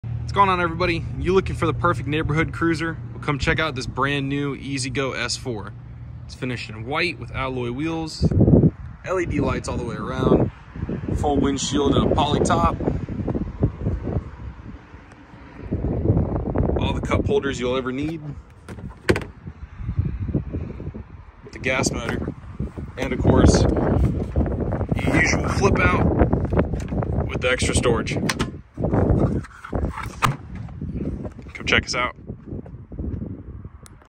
2022 E-Z-GO S4 Lifted Golf Cart
Y’all check out this brand new 2022 EZ-GO S4 available now! Come see it in person at Jack Rabbit Offroad in Marshall or Huntsville, TX

Jack Rabbit Offroad is your E-Z-GO Golf Cart Super Store for East Texas!!